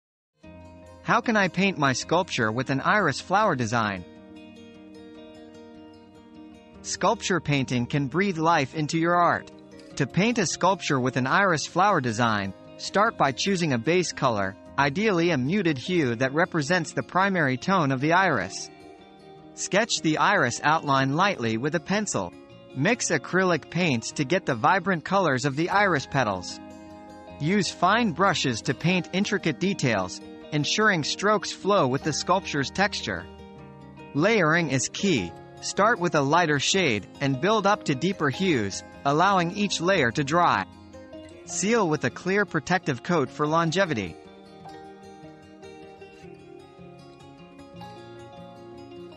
How Can I Paint My Sculpture with an Iris Flower Design?
Painting a Sculpture with an Iris Flower Design: Step-by-Step Guide • Sculpture Painting: Iris Flower Design • Discover how to bring your sculpture to life with a stunning Iris Flower design. Learn the step-by-step process, from choosing the base color to adding intricate details, and seal it for long-lasting beauty.